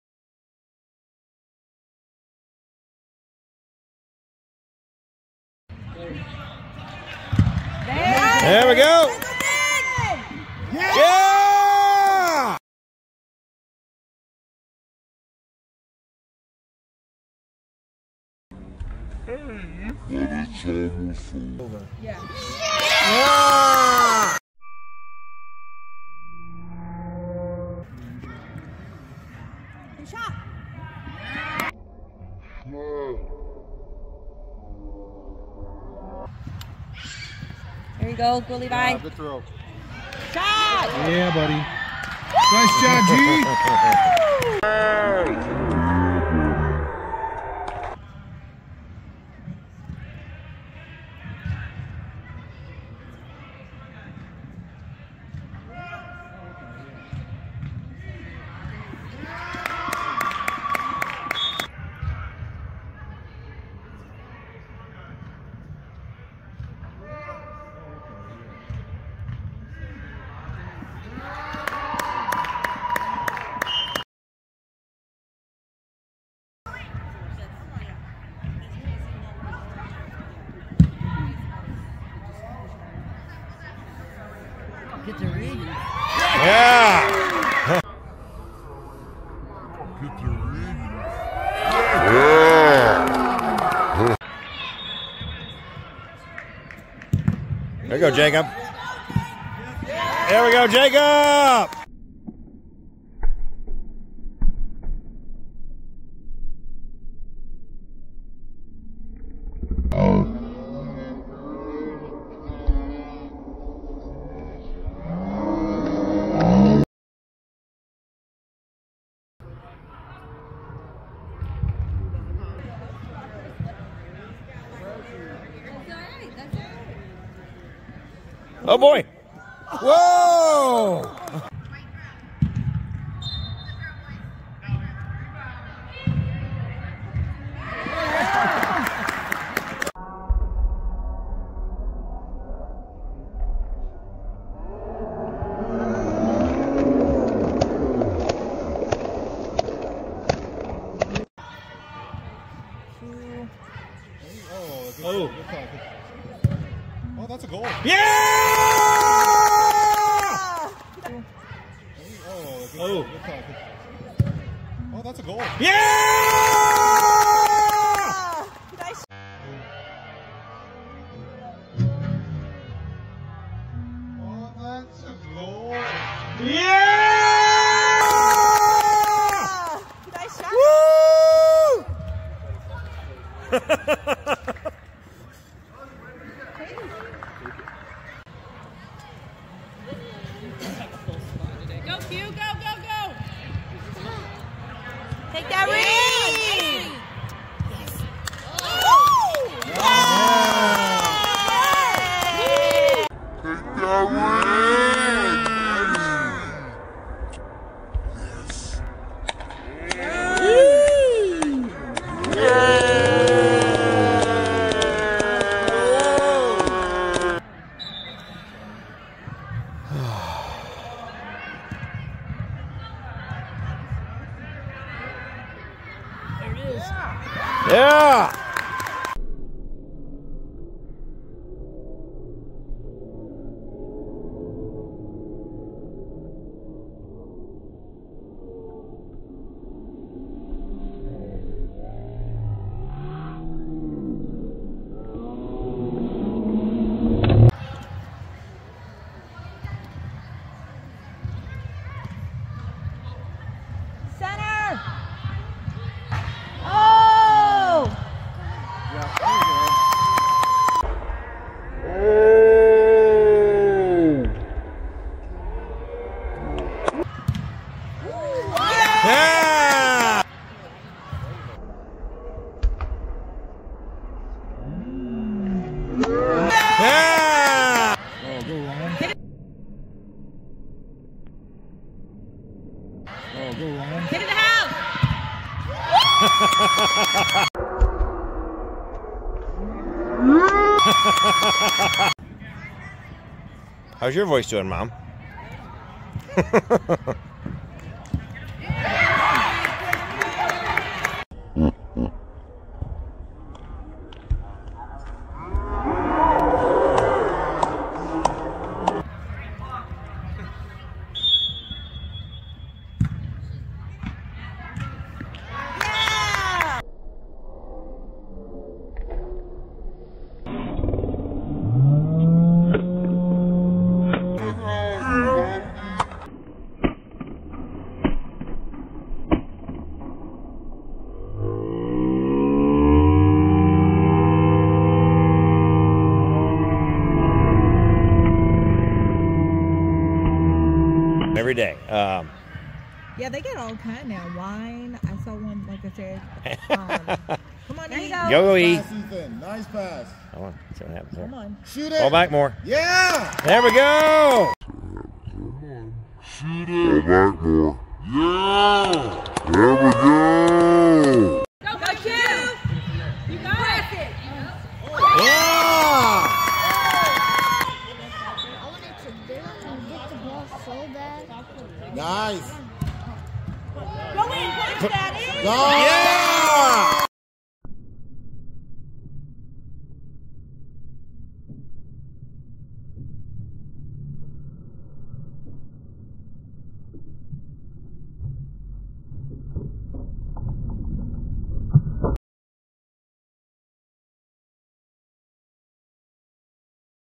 2023 - 2024 Winter Indoor GATEWAY FIRE FC 7v7
This is 7v7 Winter Indoor. We had a lot of fun!